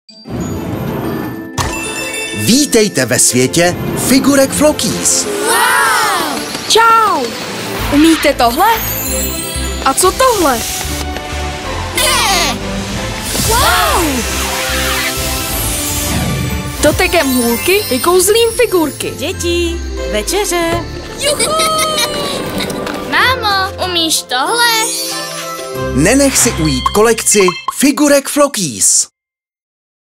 Flockies úžasná kolekce zvířecích figurek
Flockies je úžasná kolekce zvířecích figurek, které spojují zábavu se vzděláváním. V každém balení najdete jednu figurku a sběratelský letáček obsahující informace o celé kolekci a fakta o figurce (zvířeti). K sesbírání je 24 figurek (dvě z nich jsou dostupné pouze v sadě se sběratelským kufříkem)

TM Toys - Hračky přátelské k dětem - Motto firmy TM Toys vám zaručuje nejvyšší kvalitu produktů a také plno radosti a zábavy.